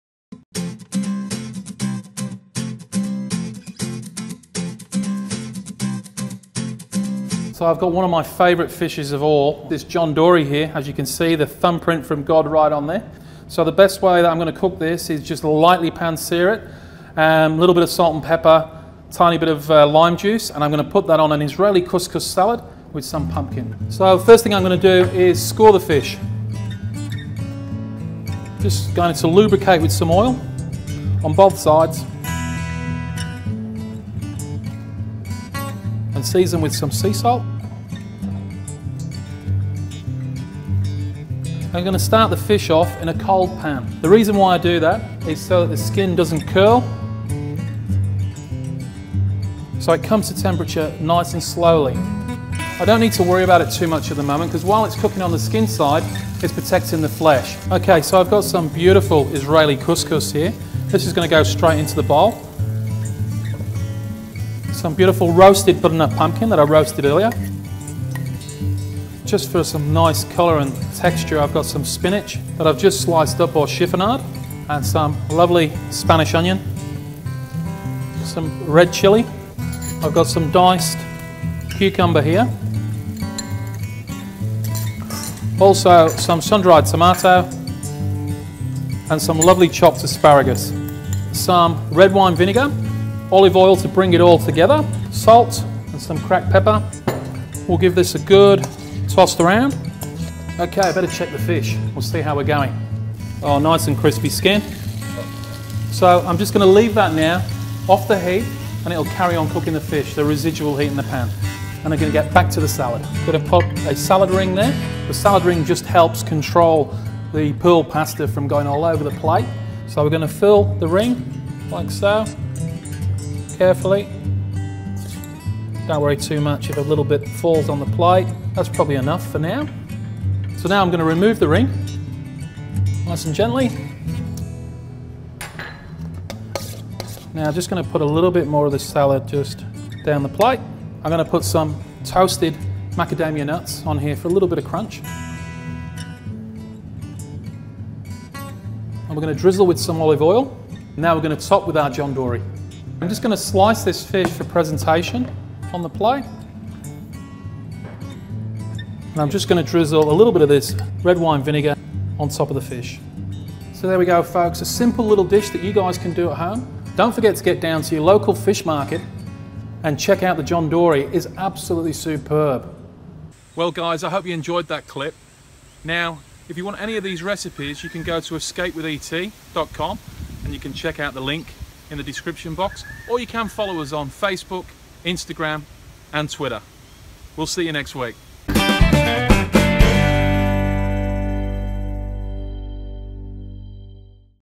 How to Cook John Dory |The Hook and The Cook| RECIPE | Paul Breheny
Paul Breheny shows you how to cook John Dory  fillet with butternut pumpkin macadamia nut spinach and Israeli  Cous Cous.
John Dory a beautiful white fleshed fished. Its said to be blessed by God himself and has the thumb print to prove it.
Paul shows you his way that he likes to cook this wonderful tasty fish. We hope you enjoy this clip. Paul has written the recipe here for you to follow:
Serves 2
Ingredients:
1. 2 x 180g portions of John dory  fillet – Skin on

1 x lime
2 red Chillies – Finely chopped
 200g Israeli cous cous
20g Persian feta optional
2. 100g baby spinach – washed and sliced
3. ½ small Spanish onion
4. 1/2 Whole butternut pumpkin – peeled, and cut into 2cm cubes
5. 1 Lebanese cucumber Diced
6. 6x Sundried tomato diced
7. Fresh asparagus blanched and diced
8. 100g macadamia nuts
Salt

Red wine vinaigrette
100mls red wine
100mls red wine vinegar
20g white sugar
Pinch sea salt
Method for red wine dressing
Combine all ingredients in a small pot and reduce until slightly thick allow cooling. The liquid will thicken once cool .
Once cool add extra virgin olive oil.
 Method:

Preheat an oven to 250°C

Cut pumpkin into cubes roughly two centimetres by two centimetres, toss in olive oil and season generously with sea salt and cracked black pepper. Place cut pumpkin onto a baking tray with the whole garlic cloves and roast until just cooked, reduce temperature to 200°C and continue to roast until crisp and light golden brown in colour. Reserve and keep warm.
Prepare Israeli or pearl couscous, you'll need about 1 1/4 cups of water or stock for every 1 cup of dry grain. Simmer the grains stovetop, covered, for about 10 minutes. The grains fluff up just slightly and have more an "al dente" mouth feel when cooked.

• Slice the Dory skin on the fillet this helps stop the fish from curling.
•  In a cold pan place the John Dory Fillets skin side and slowly increase the heat without moving for approximately 4 to 5minutes or until the skin is crisp.
• Turn the fillets over and fry for another three minutes and turn off heat and rest in a warm place.
• Cut macadamia nut into quarters and toast lightly to release oils.
• Strain the Israeli cous cous    and place in a bowl with the baby spinach, chilli, Roasted butternut pumpkin, diced red onion, cucumber asparagus and sundried tomato.
• Toss and add red wine vinaigrette
• Arrange on plate
• Add John Dory fillets and scatter around some macadamia nuts.
• Top salad with fetta but this is optional.

Hope you enjoy this video and learn a few tips & tricks along the way. Thanks Regards Scotty & Paul.